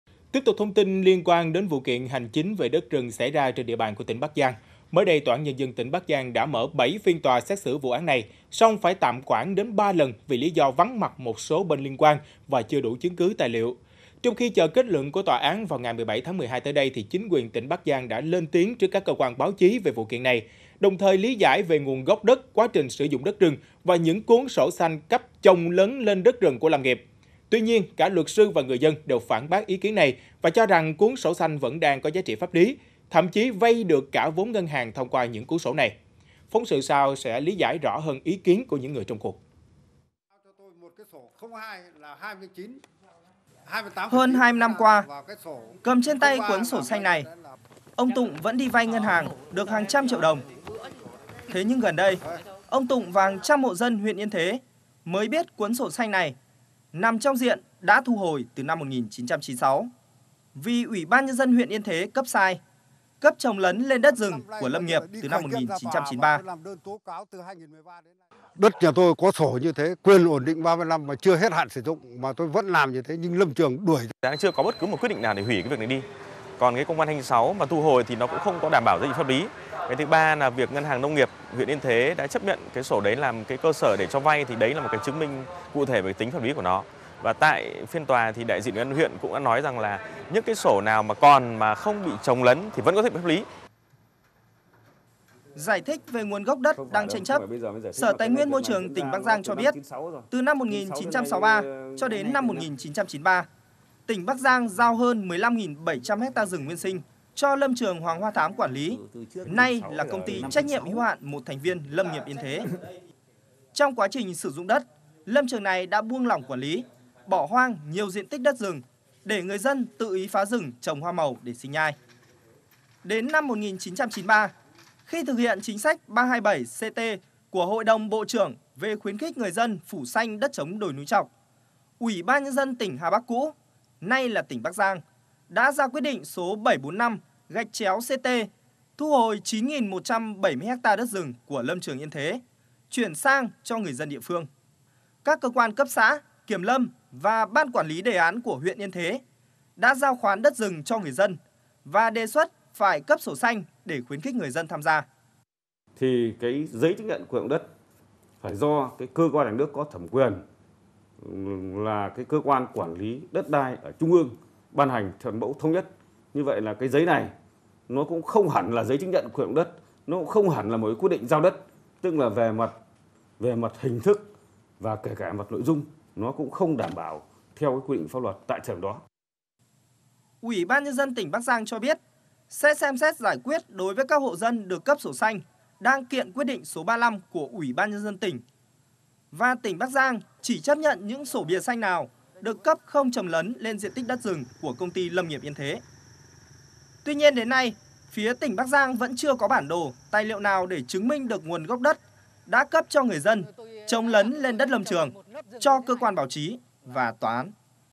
Bắc Giang: Tranh cãi về tính pháp lý sổ xanh đã cấp cho các hộ dân | VTC16
VTC16 | Trong khi chờ kết luận của tòa vào ngày 17/12  tới. Chính quyền tỉnh Bắc Giang đã lên tiếng trước các cơ quan báo chí về vụ kiện này. Đồng thời lý giải về nguồn gốc đất, quá trình sử dụng đất rừng và những cuốn sổ xanh cấp chồng lấn lên đất rừng của lâm nghiệp. Tuy nhiên cả luật sư và người dân đều phản bác ý kiến này và cho rằng cuốn sổ xanh vẫn đang có giá trị pháp lý, thậm chí vay được cả vốn ngân hàng thông qua những cuốn sổ này.